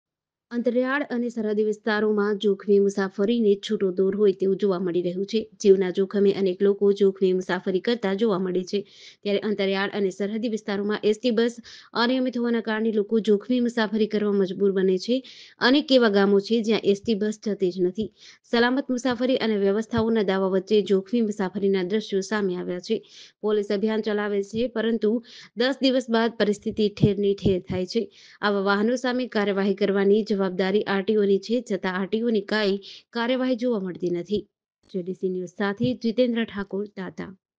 અંતરિયાળ અને સરહદી વિસ્તારોમાં જોખમી મુસાફરીને છૂટો દોર
જીવના જોખમે અનેક લોકો જોખમી મુસાફરી કરી રહ્યા છે